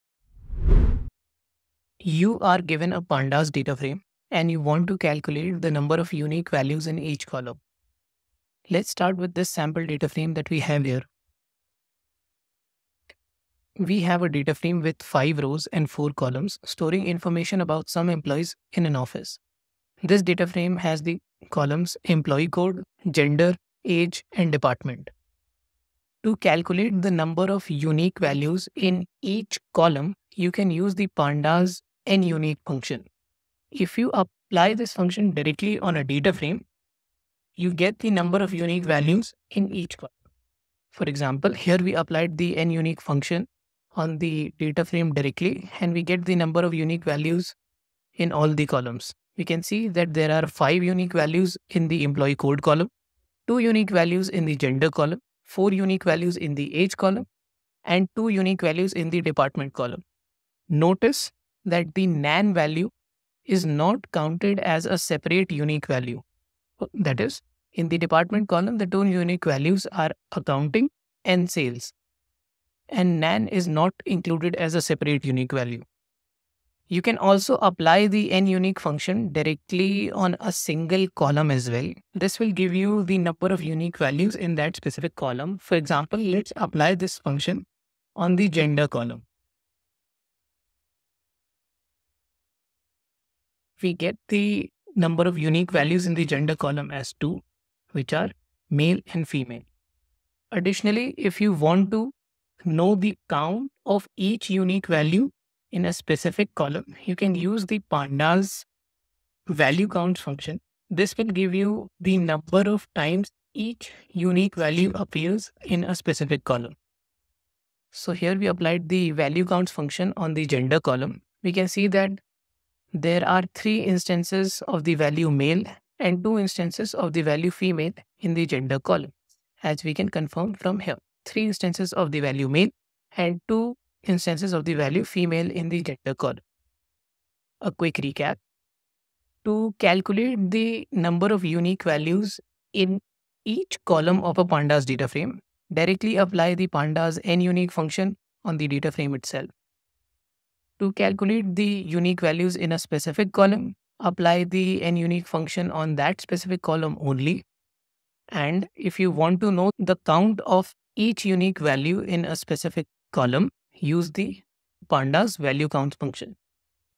Count Unique Values in Each Column in Pandas | Python Tutorial
How do you count the number of unique values in each column of a Pandas Dataframe in Python?

In this tutorial, we explore how can we get the distinct value count of each column in a Pandas Dataframe . Watch this video for a complete step-by-step approach along with code and relevant outputs.